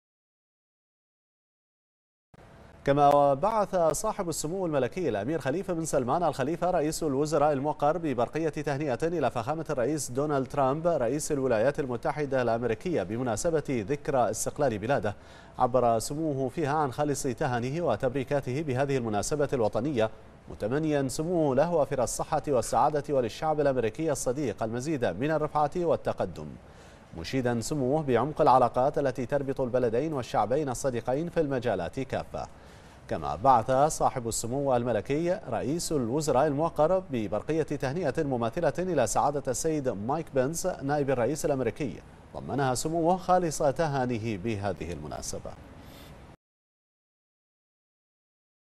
البحرين: سمو رئيس الوزراء يبعث برقية تهنئة إلى رئيس الولايات المتحدة بمناسبة ذكرى استقلال بلاده
المنامة في 4 يوليو /بنا/ بعث صاحب السمو الملكي الامير خليفة بن سلمان ال خليفة رئيس الوزراء الموقر حفظه الله ورعاه برقية تهنئة الى فخامة الرئيس دونالد ترامب رئيس الولايات المتحدة الامريكية وذلك بمناسبة ذكرى استقلال بلاده، عبر سموه فيها عن خالص تهانيه وتبريكاته بهذه المناسبة الوطنية، متمنيا سموه لفخامته وافر الصحة والسعادة وللشعب الأمريكي الصديق المزيد من الرفعة والتقدم، مشيدا سموه بعمق العلاقات التي تربط البلدين والشعبين الصديقين في المجالات كافة.